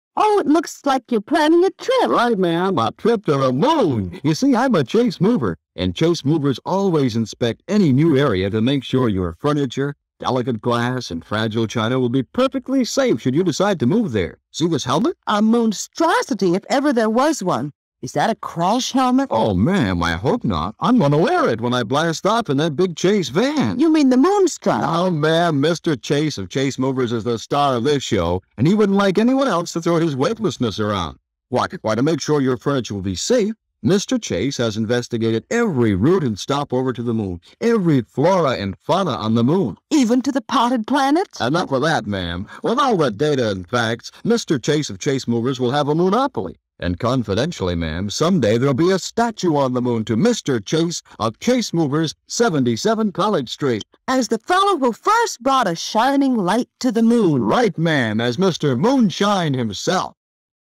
Vintage Radio Spot: Chase Movers "Moon Shot" 1960s
Here at our secret underground base, we're always working to unearth interesting recordings. This one is from a reel to reel found in a box purchased an estate sale,  which contains this series of radio spots for Chase Movers, of Burlington VT. The undated and unlabeled reel seems to be a dub, and was quite oversaturated as you'll hear.

They definitely don't make commercials like these any more!
We estimate that the date is the recording is sometime in 1960s.